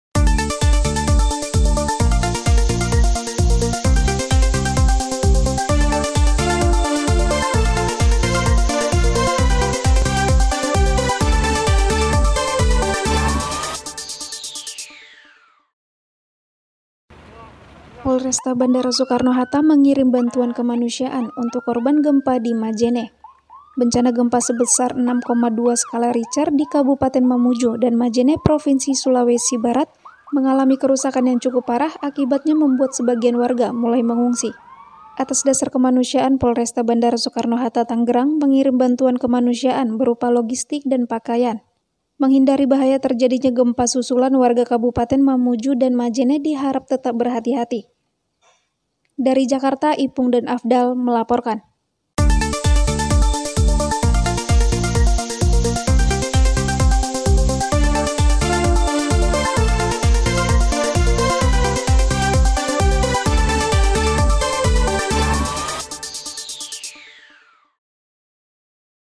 POLRESTA BANDARA SOETTA KIRIM BANTUAN BAGI KORBAN GEMPA DI MAMUJU - MAJENE
Polresta Bandara Soekarno – Hatta mengirim bantuan kemanusiaan untuk korban gempa di Majene.

Bencana gempa sebesar 6,2 skala ritcher, di Kabupaten Mamuju dan Majene Provinsi Sulawesi Barat mengalami kerusakan yang cukup parah.
Akibatnya membuat sebagian warga mulai mengungsi. Atas dasar kemanusiaan Polresta Bandara Soekarno – Hatta Tangerang, mengirim bantuan kemanusiaan berupa logistik dan pakaian.

Menghindari bahaya terjadinya gempa susulan. Warga Kabupaten Mamuju dan Majene diharap tetap berhati–hati.

Tim liputan : Ipung dan Afdal
Melaporkan.